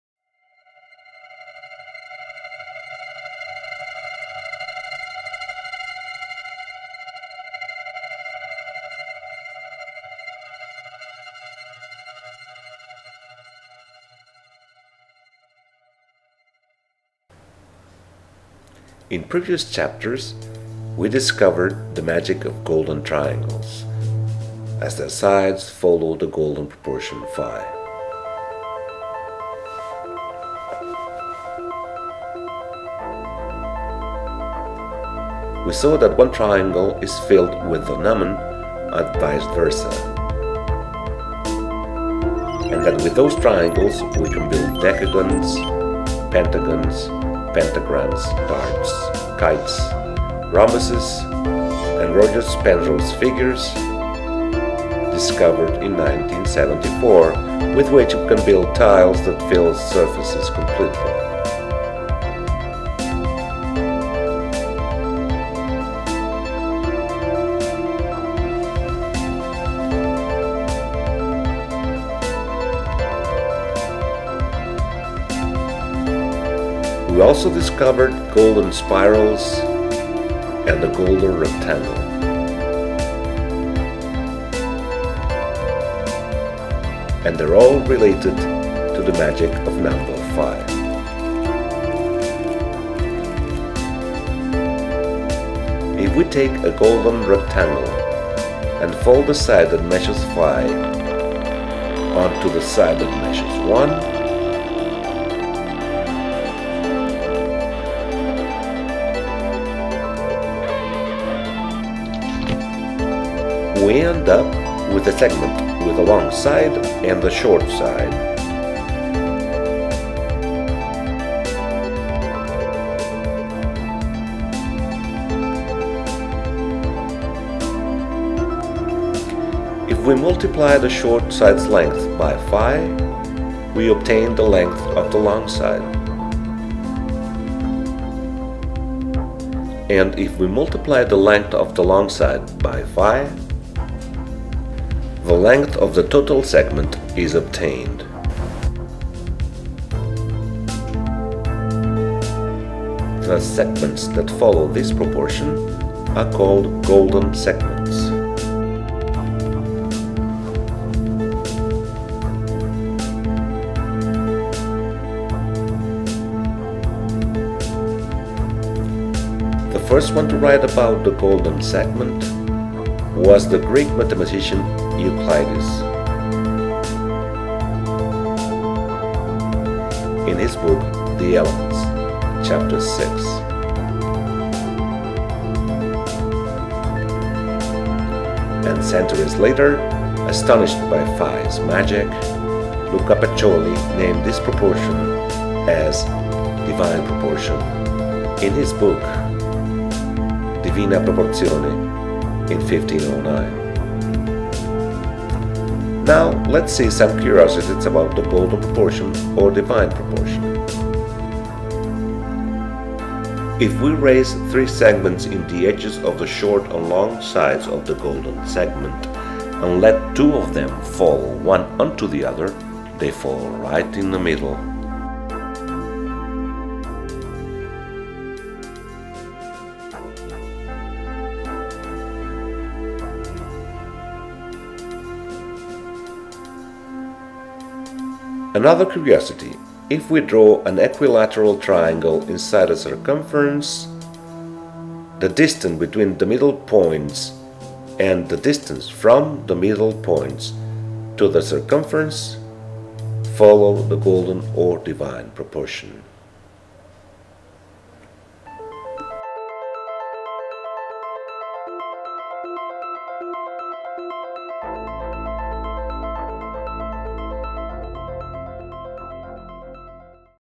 There's something about phi - Chapter 4 - The golden segment and the divine proportion
Chapter 4 of the series "There's something About phi". Video on the number phi, golden number, divine proportion or golden ratio and its relationship with the golden segments. Also two geometrical curiosities about golden segments.